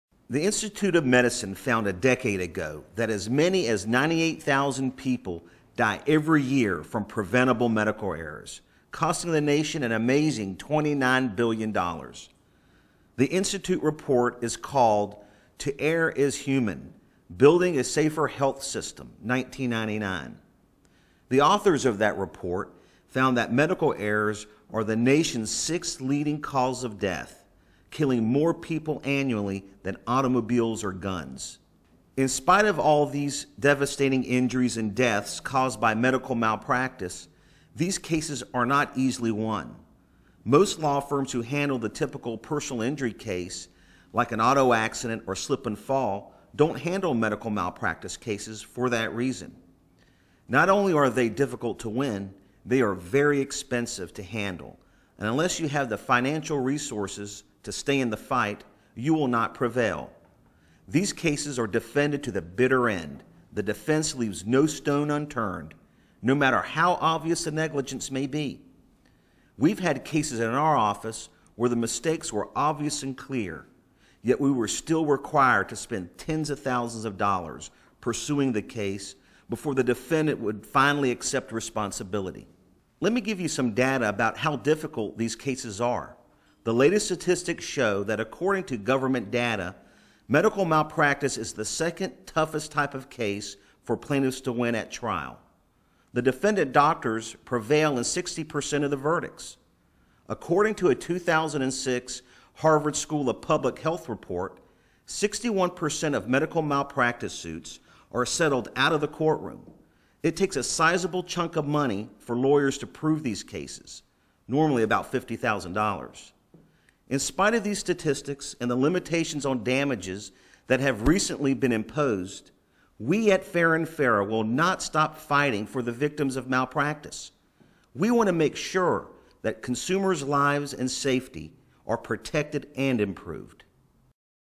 Medical Malpractice Statistics | Farah & Farah
According to a study done in 1999, medical errors are the sixth leading cause of death. Medical malpractice cases are one of the hardest cases to win for plaintiffs, so many other law firms have stopped taking those cases. Farah & Farah has not stopped, and we will continue to fight for malpractice victims until they get their justice.